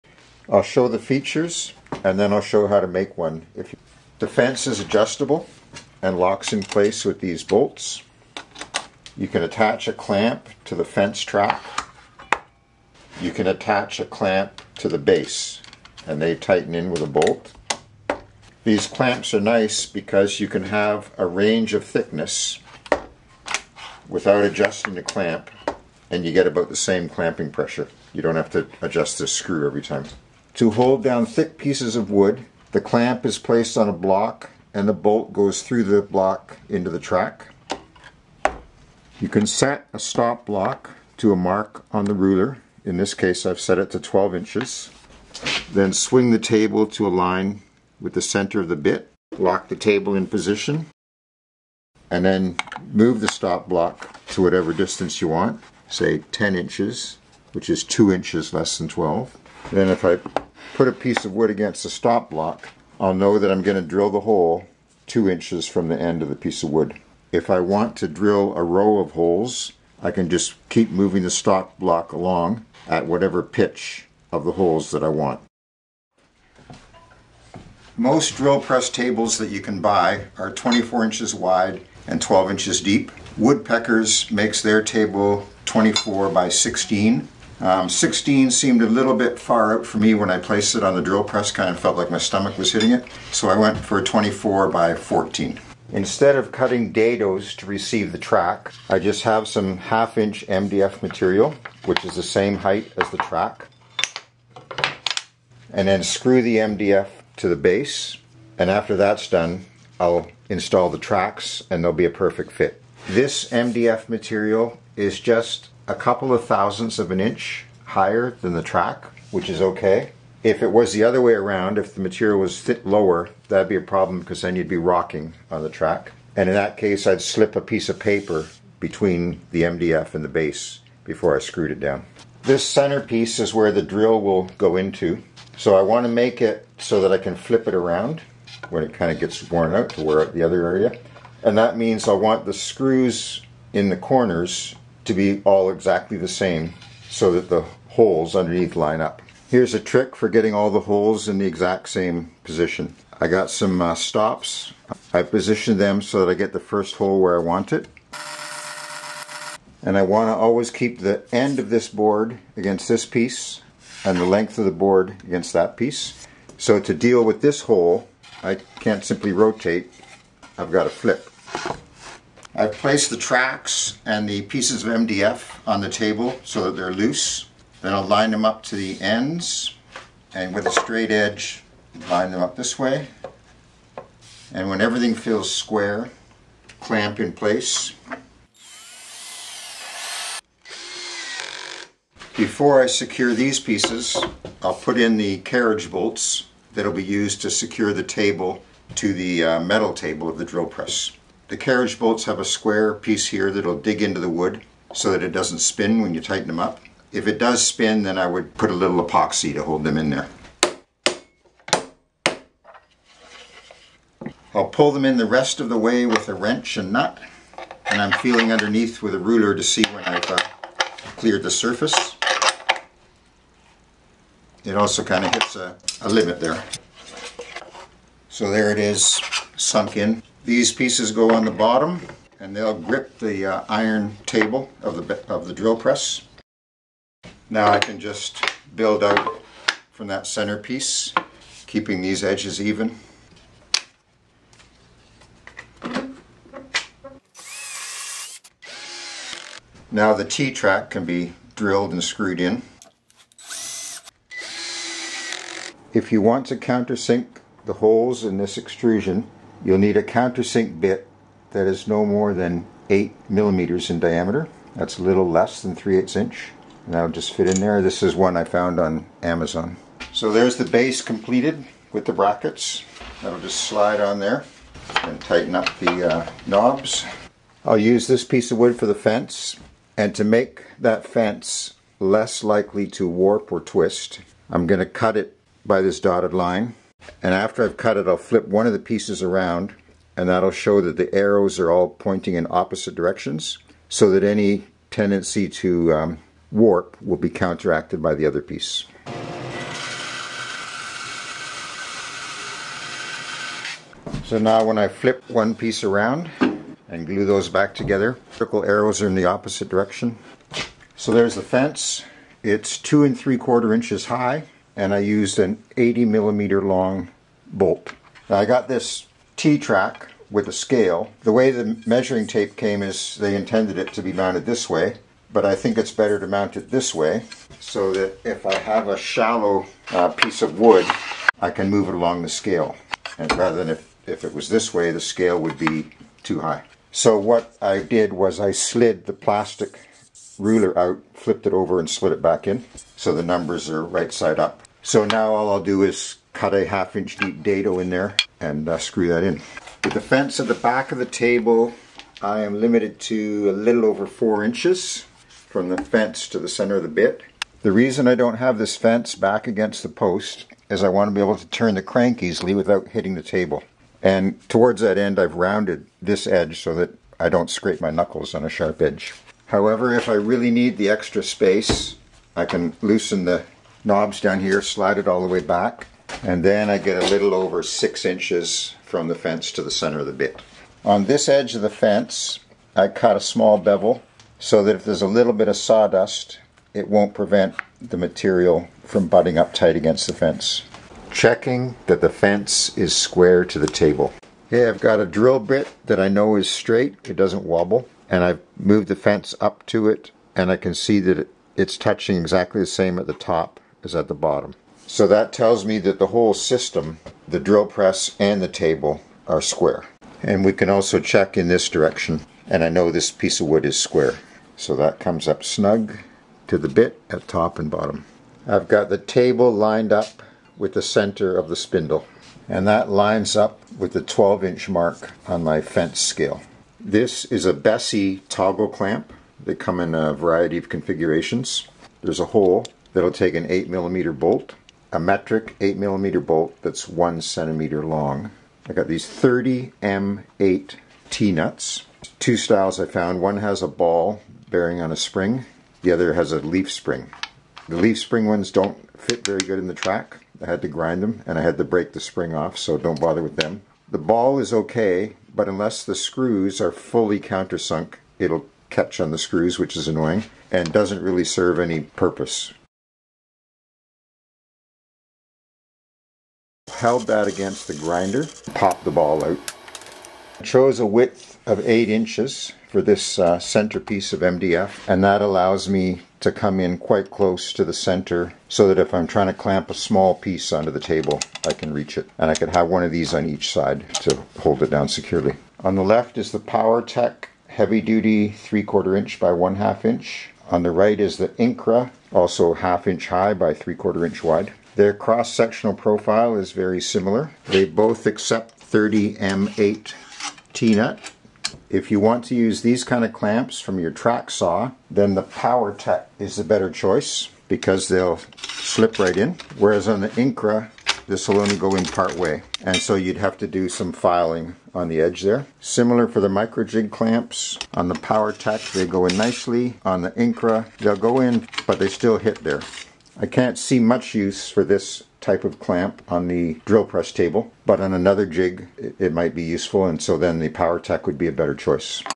Drill Press Table
Make a table for drill press using T-tracks from Incra or Powertec, and Bessey toggle clamps

Chapters
00:00   Features
01:18   Make it
04:37  Fence
06:31  Quality checks
08:05  Extrusion comparison